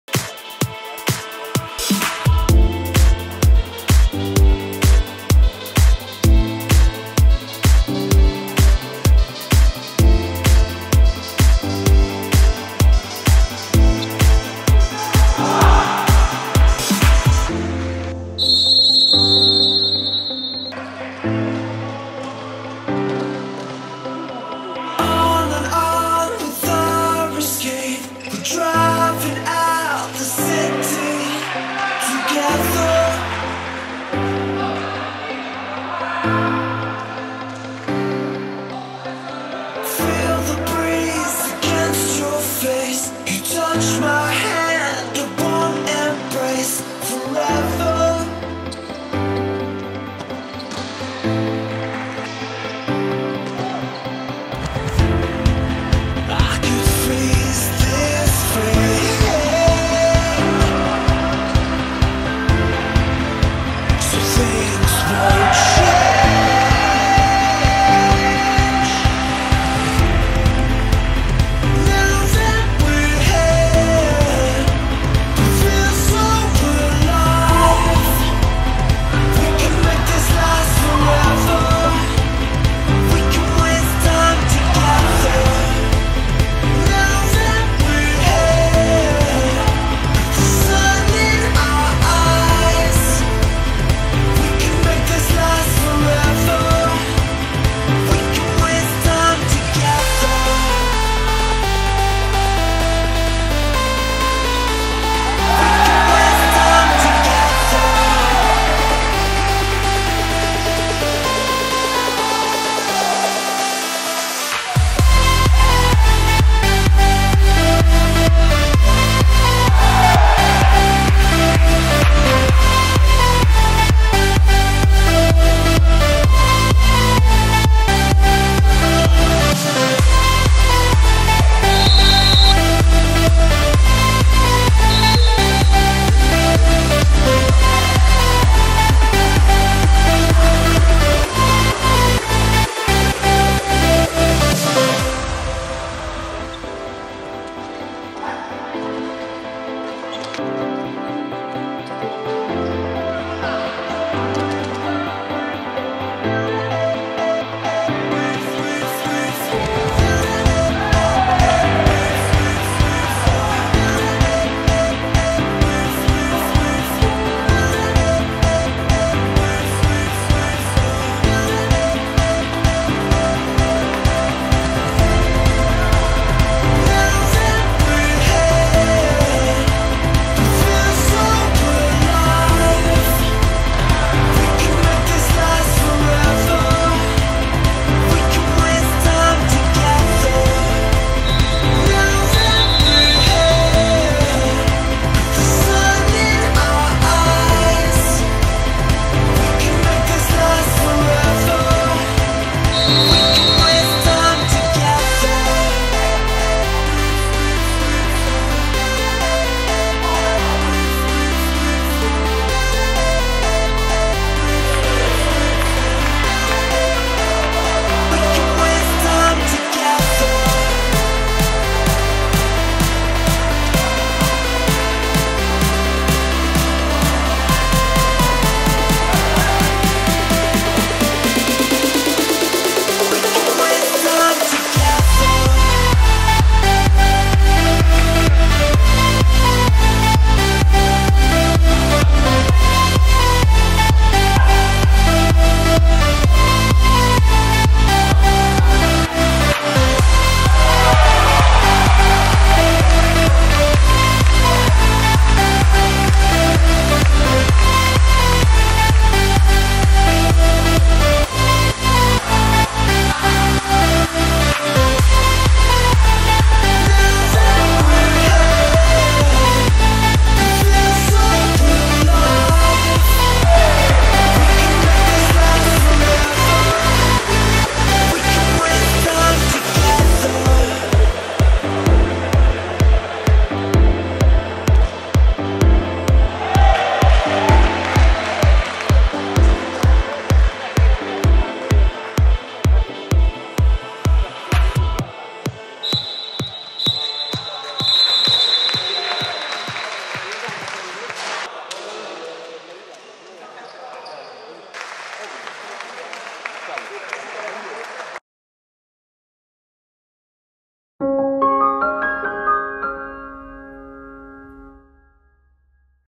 Futsal Cobà 11 - 1 Adverso
Vittoria schiacciante per il Futsal Cobà in quest'ultima giornata di campionato. Gli Sharks si sono imposti senza troppe difficoltà su di una Adverso che ha faticato nel trovare la giusta reazione alle scorribande sangiorgesi. Una produzione Morese Studios©